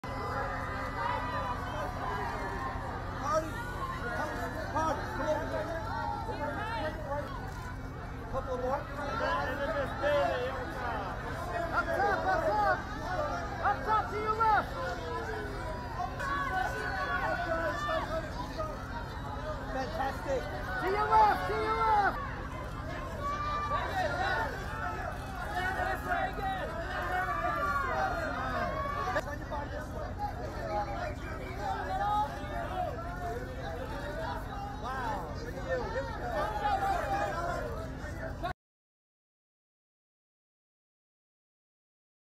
Cardi B glitters in gold at Met Gala
Cardi B, Ariana DeBose, Megan Thee Stallion and Michaela Jaé Rodriguez shined bright in gold and glitter at the Met Gala. (May 2)